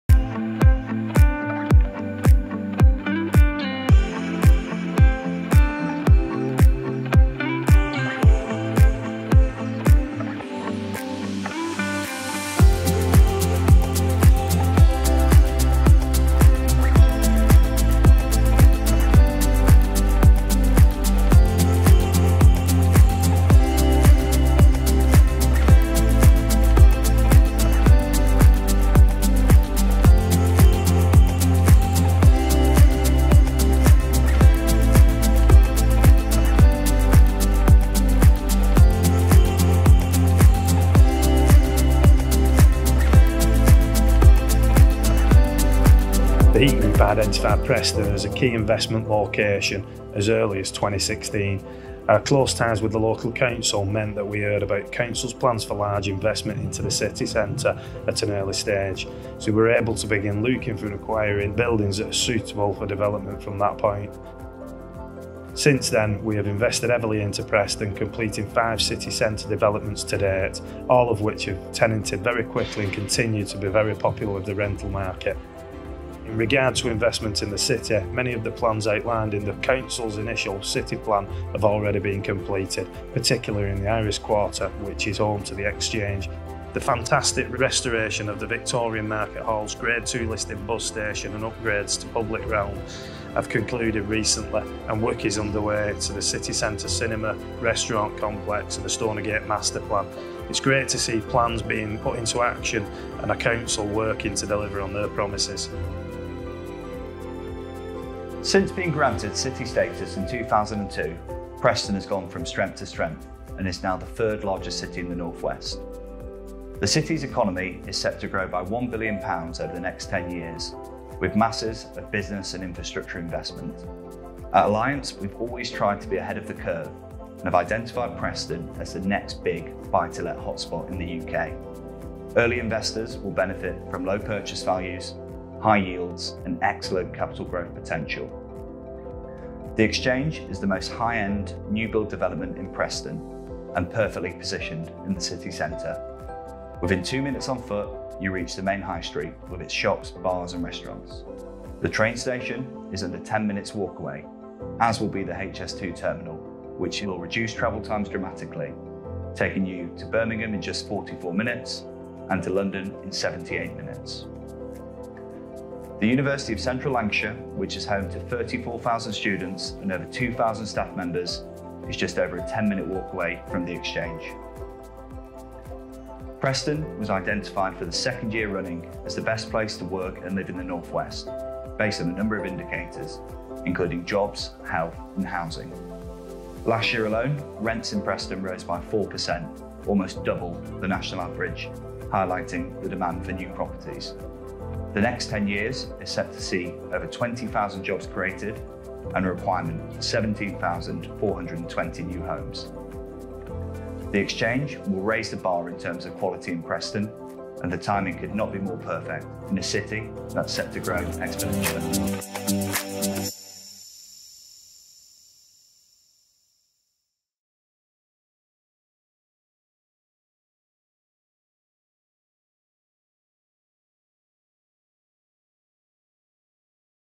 The Exchange, Preston - Luxury apartments to buy
Open the door to a new way of life at The Exchange in Preston, Lancashire. Offering an outstanding level of facilities, interiors and communal areas, this stunning property development provides residents with some of the best accommodation in the area. This prestigious development boasts stylish new build apartments with spacious layouts that focus on contemporary design and high-quality living, set within an impressive building with cutting-edge facilities. The development can be found in the heart of Preston City Centre and offers easy access to a wide variety of amenities.